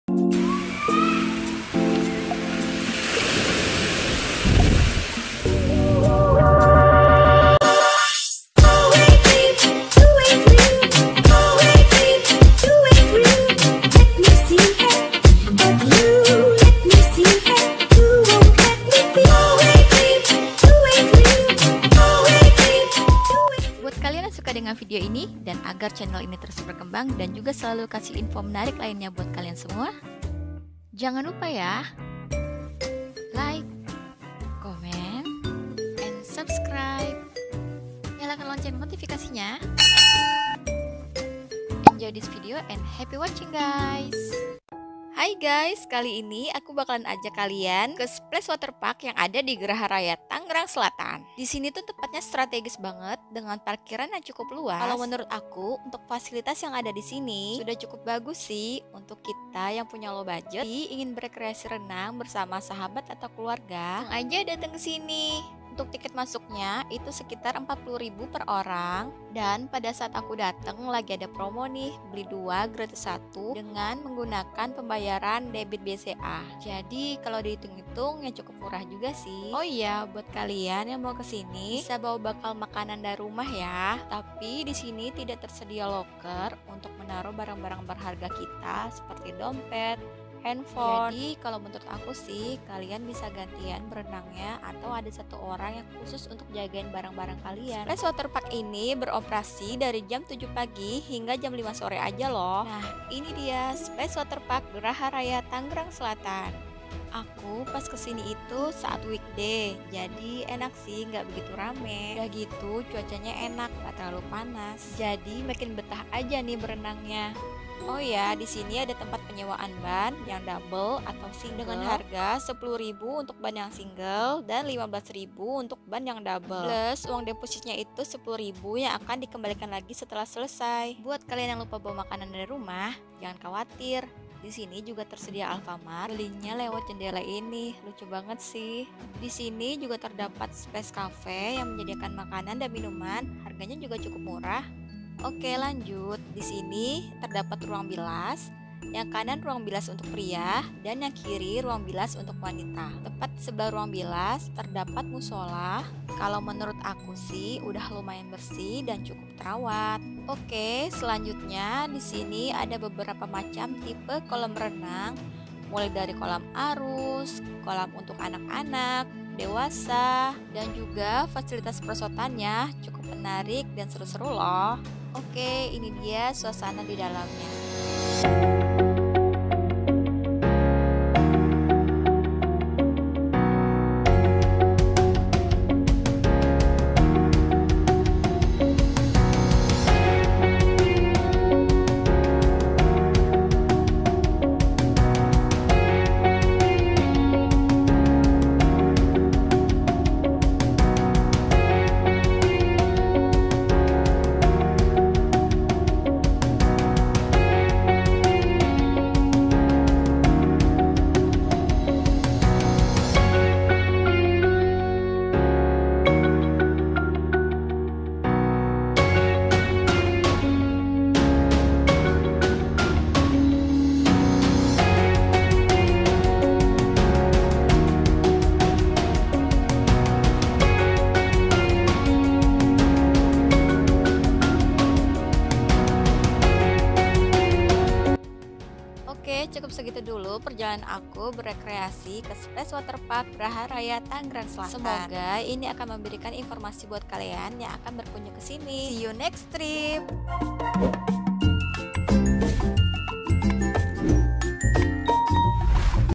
Review Splash Waterpark Graha Bintaro || Tangerang Selatan
Hii Guyyss 👋👋

Dividio kali ini aku kasih review keseruan renang di waterpark tangerang selatan

🍃 Editing video : kinemaster
🍃 Editing thumbnail : inshot
🍃 Stiker : picsart
🍃 Background : pinterest